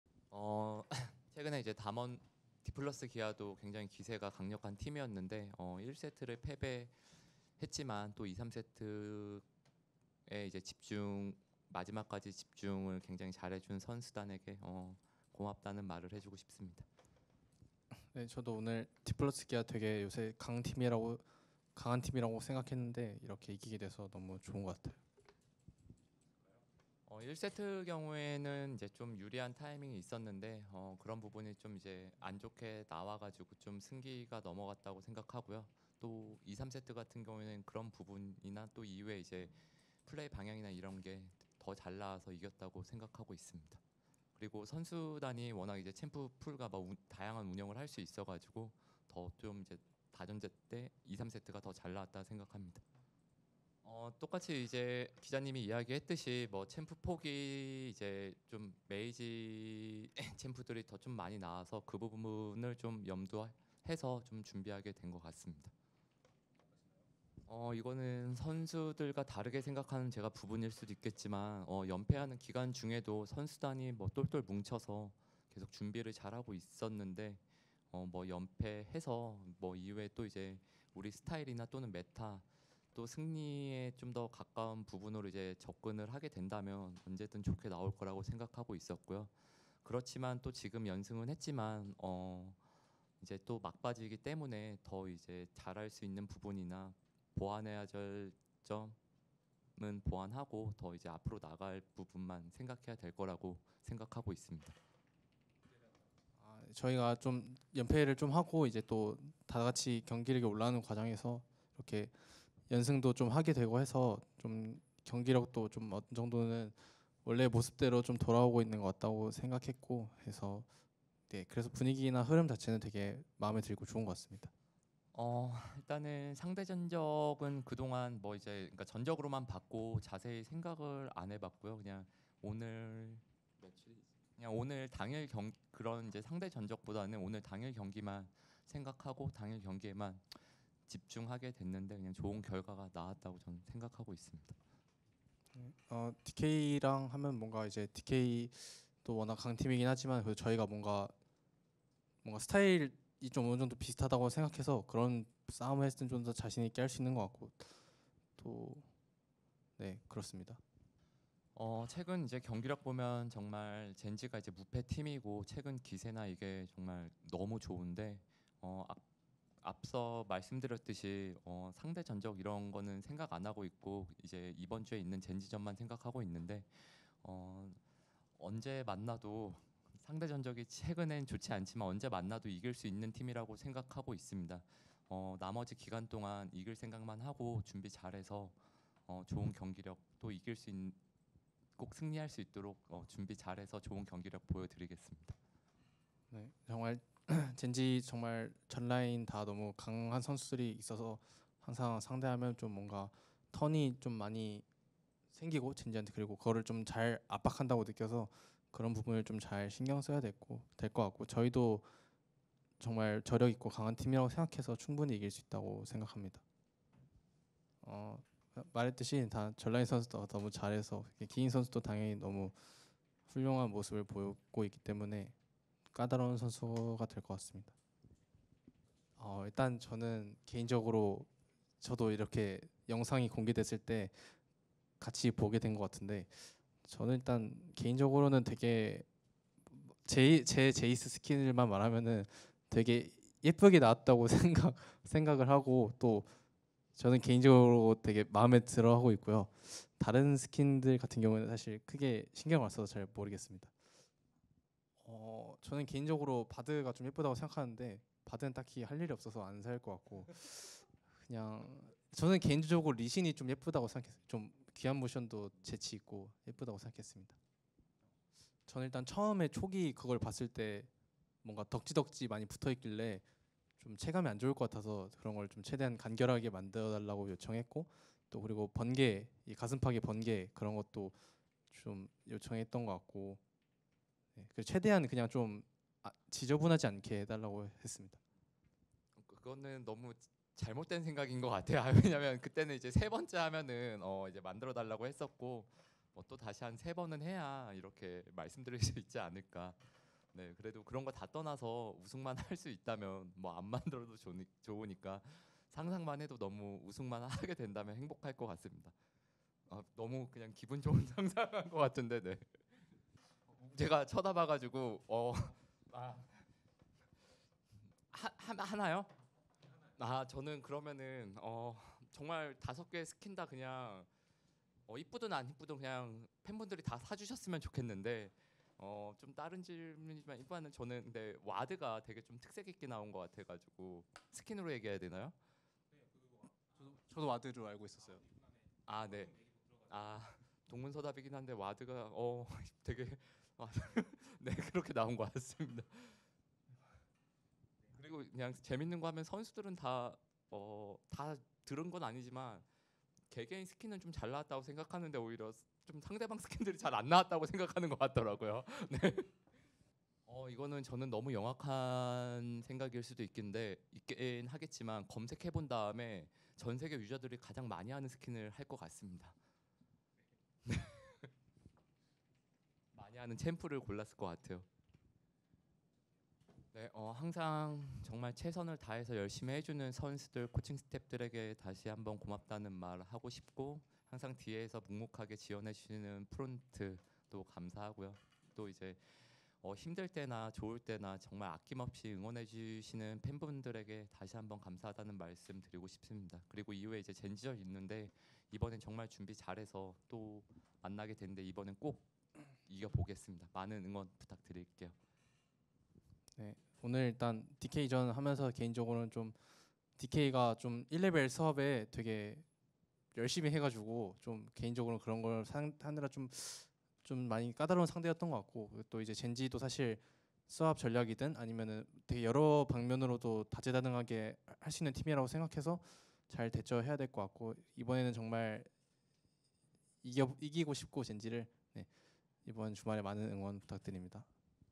[LCK 현장 인터뷰] T1 김정균 감독 "토요일 젠지전 이번에는 꼭 이겨보겠다"
2024년 7월 31일 lck 승리팀 인터뷰 T1 김정균 감독 '제우스' 최우제